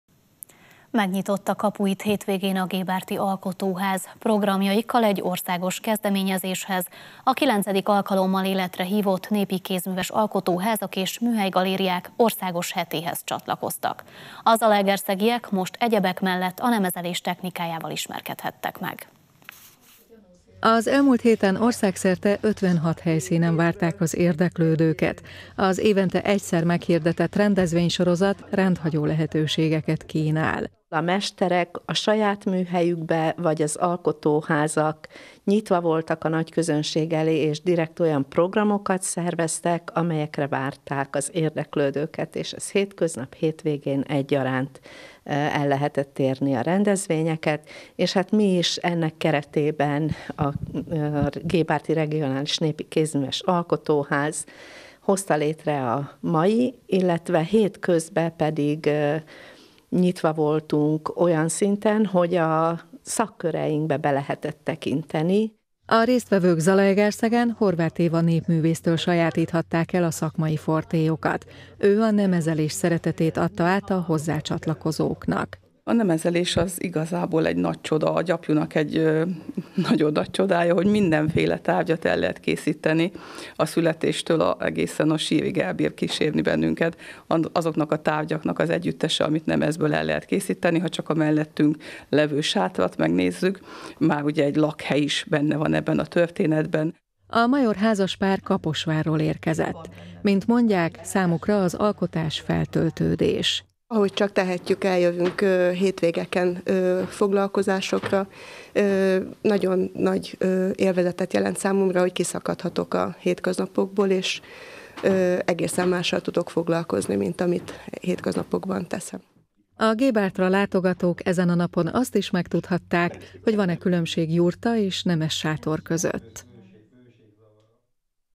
Él a ház - gébárti alkotóhétvége – ZTV Híradó 2022-04-06
Megnyitotta kapuit hétvégén a Gébárti Alkotóház. Programjaikkal egy országos kezdeményezéshez, a kilencedik alkalommal életre hívott Népi Kézműves Alkotóházak és Műhelygalériák Országos Hetéhez csatlakoztak. A zalaegerszegiek most - egyebek mellett - a nemezelés technikájával ismerkedhettek meg.